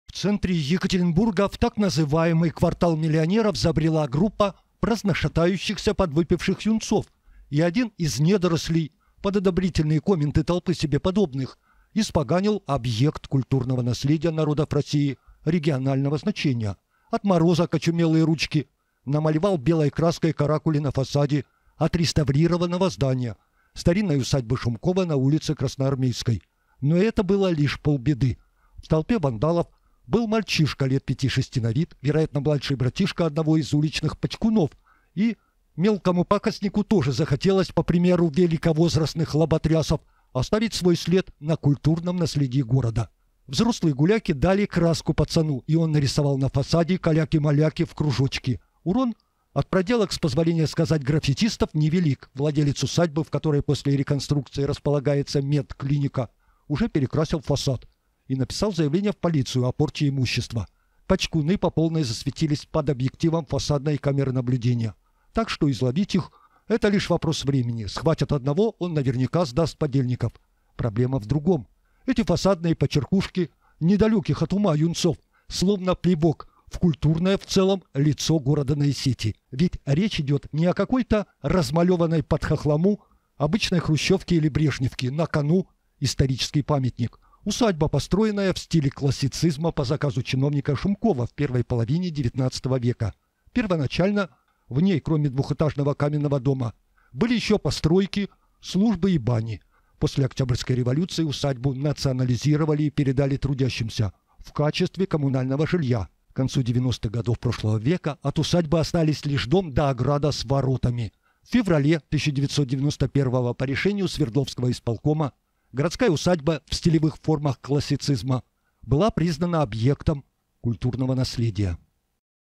Пачкуны испоганили культурное наследие
В "квартале миллионеров" юнцы расписали фасад старинной усадьбы и дали краску для "почеркушек" мелкому пацану: вандалы засветились и полиция идёт по их следу